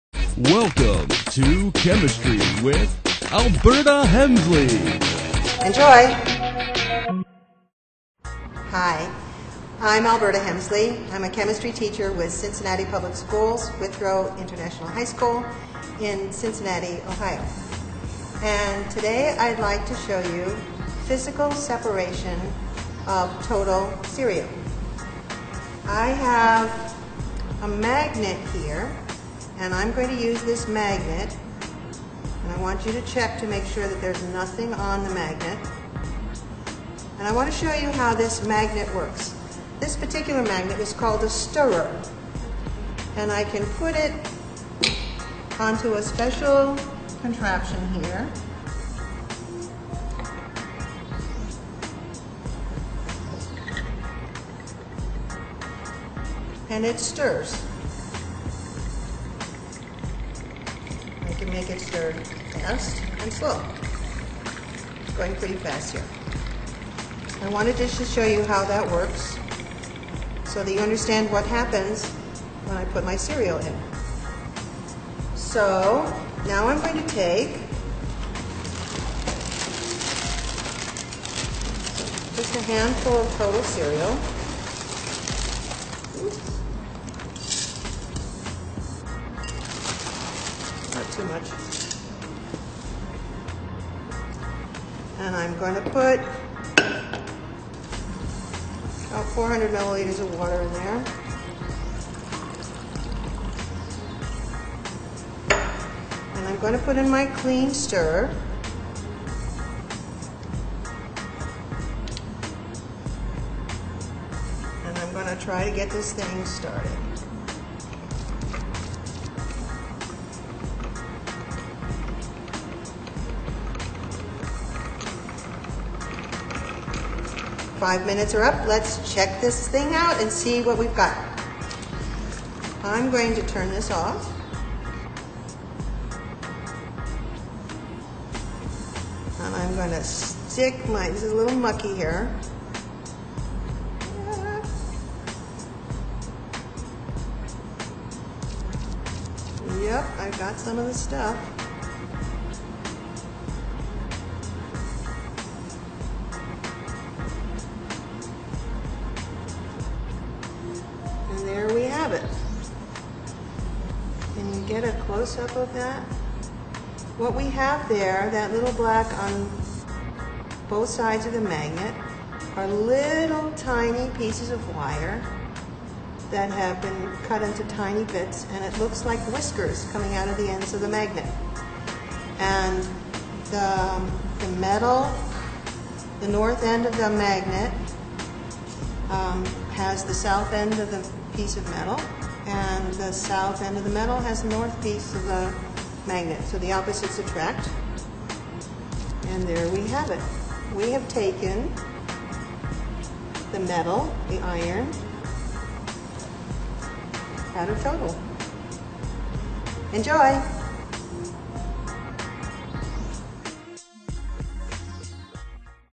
Chemistry -- Physical Separation of Cereal
Add water and a stirring magnet to a portion of Total Cereal in a 600 ml beaker and mix for a half hour or more.  See the the iron “whiskers” come out on the two ends of the magnet.

This is a physical change.  iron can be taken out of a Total Cereal mixture with a magnet without changing the chemical makeup of the ingredients.